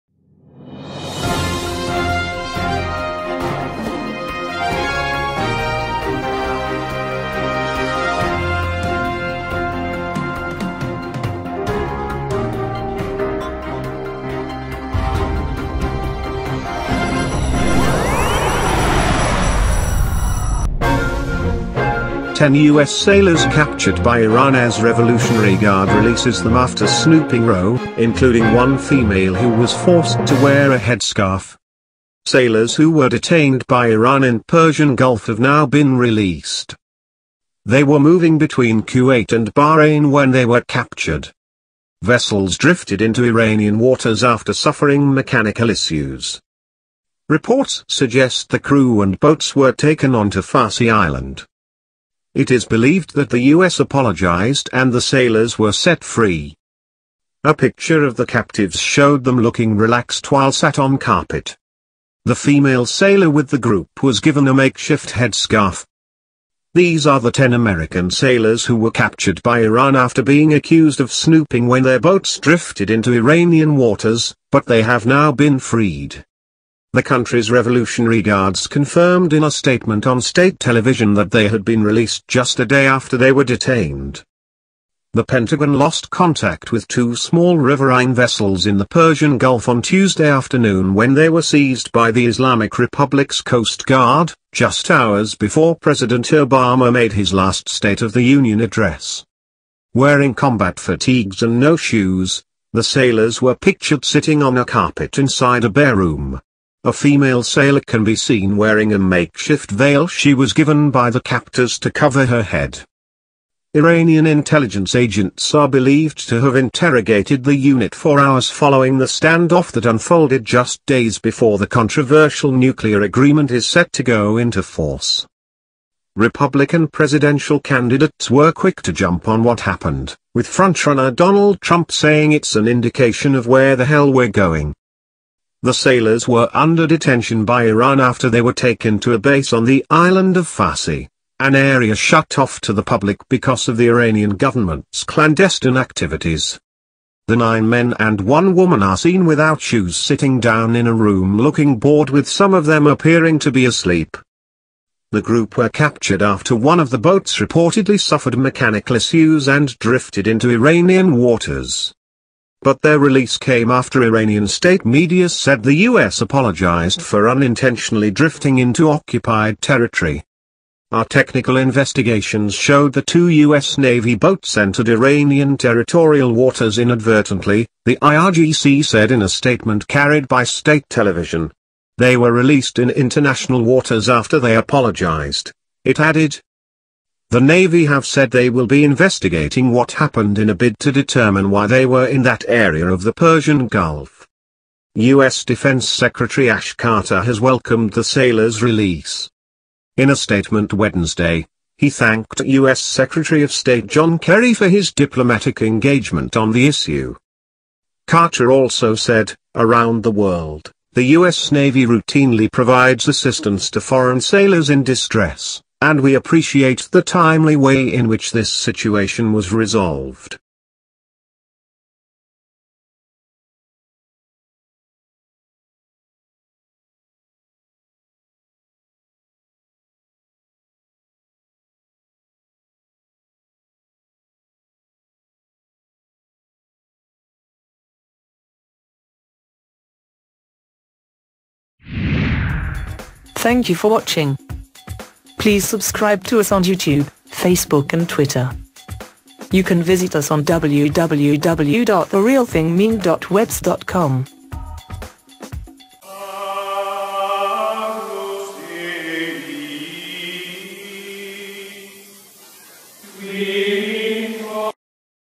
Ten U S sailors captured by Iran as Revolutionary Guard RELEASES them
Ten U.S. sailors captured by Iran as Revolutionary Guard RELEASES them after 'snooping' row - including one female who was forced to wear a headscarf
Sailors who were detained by Iran in Persian Gulf have now been released
They were moving between Kuwait and Bahrain when they were captured
Vessels drifted into Iranian waters after suffering mechanical issues
Reports suggest the crew and boats were taken onto Farsi Island
It is believed that the U.S. apologized and the sailors were set free
A picture of the captives showed them looking relaxed while sat on carpet
The female sailor with the group was given a makeshift head scarf